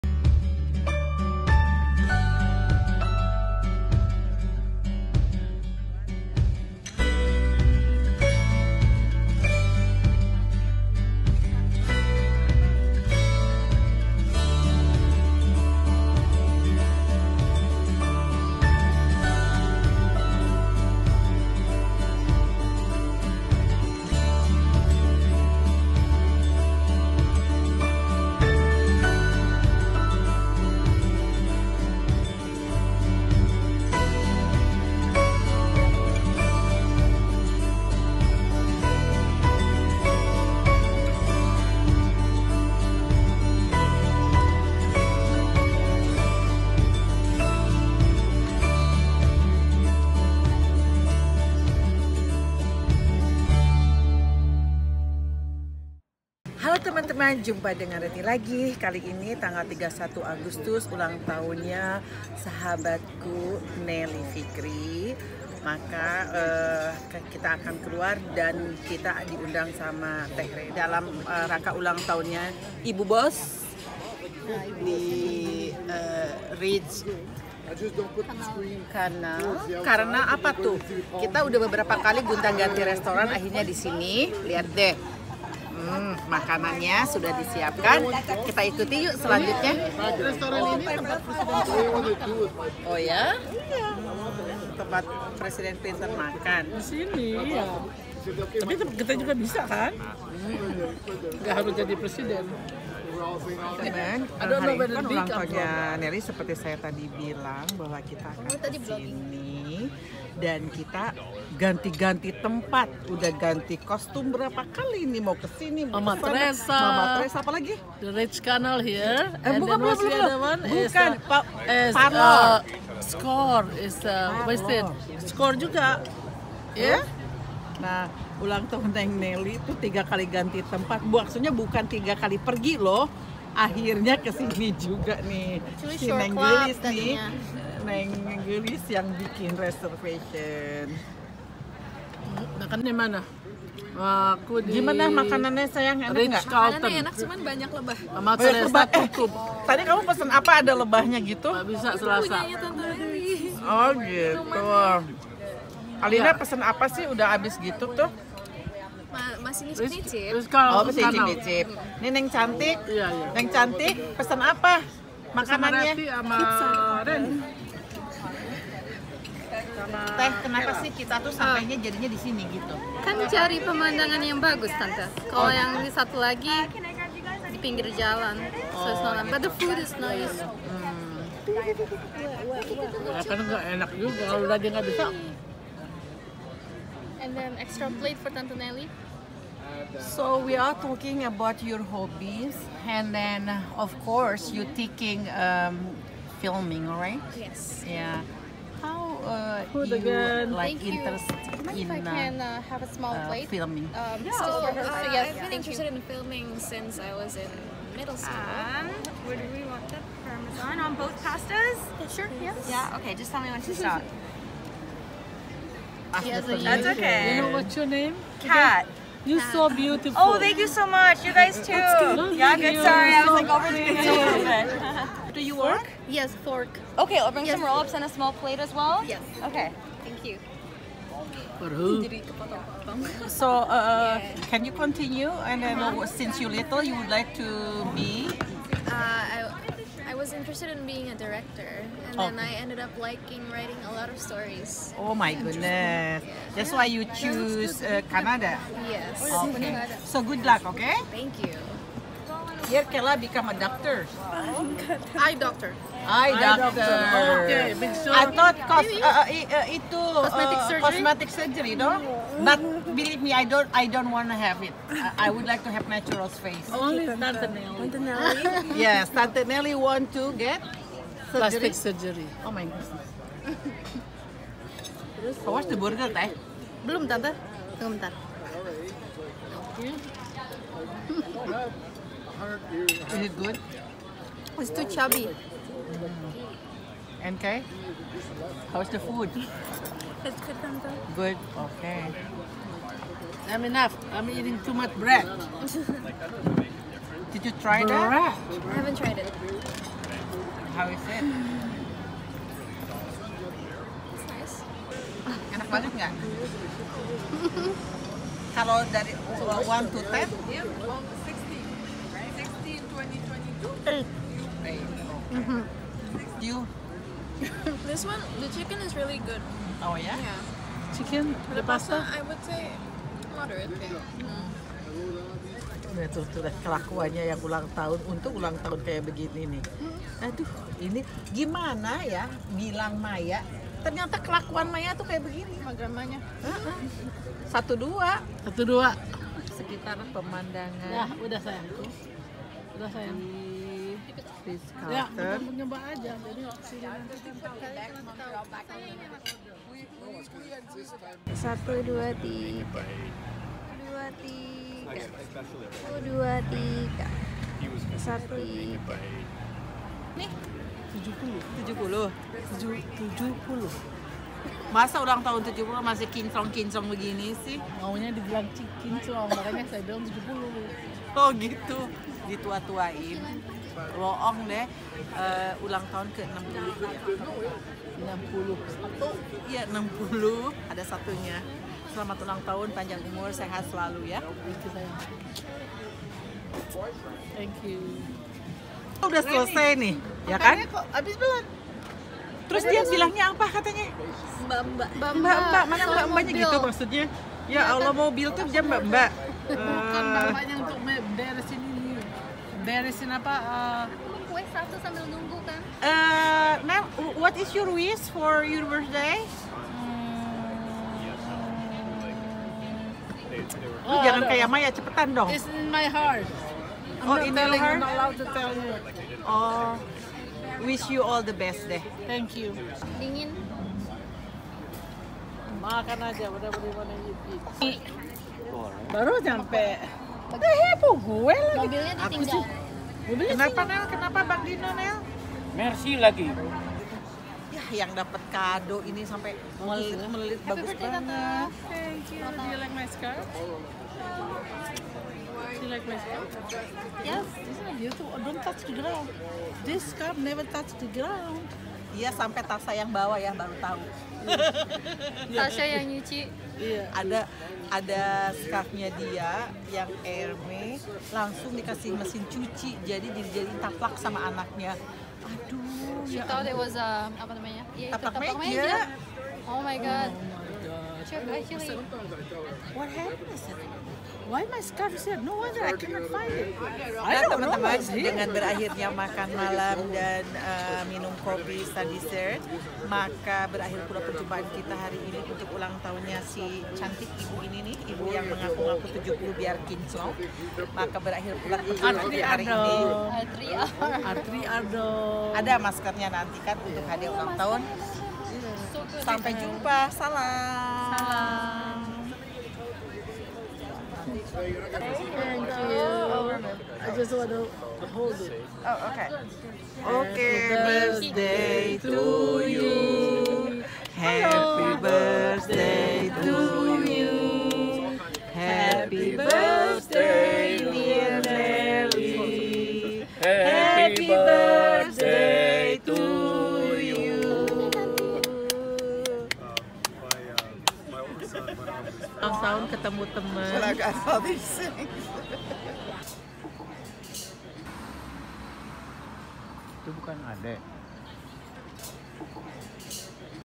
Happy birthday 🥳 🥰🎂💕@Ibu Boss @Canal Ritz Restaurant
Tante Nellie adalah Boss nya anak2, hari ini usianya bertambah semoga Allah SWT berikan Sisa usia yang baroqah Aamiin YRA 🤲🤲🤲🤲